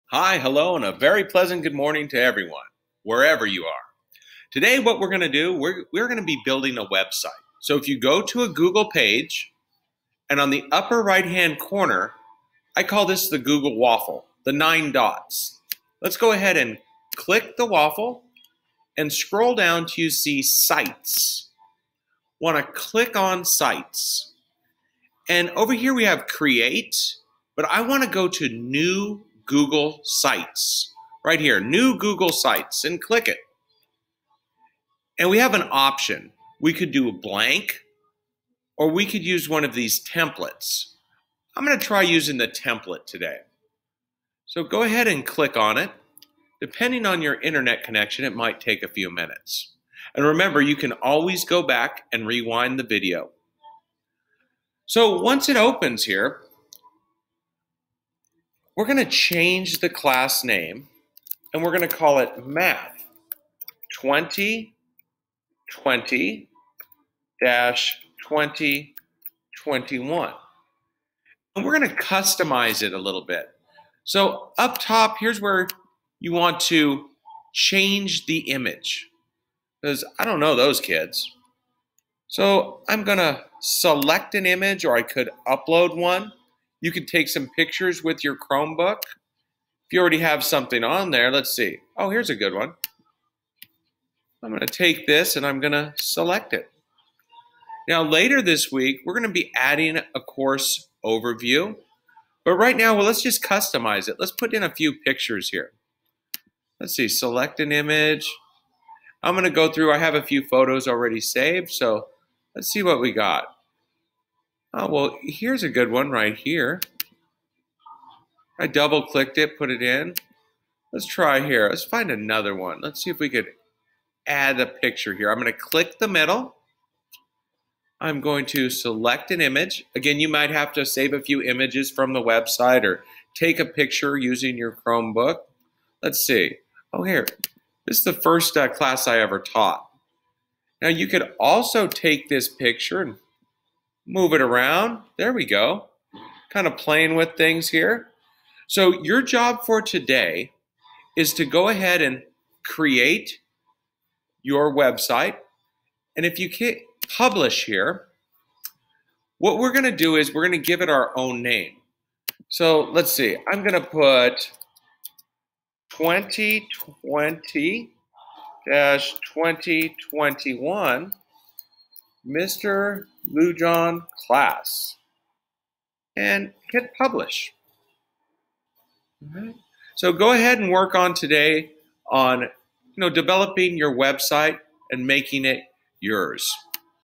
create website
Using a google template to create website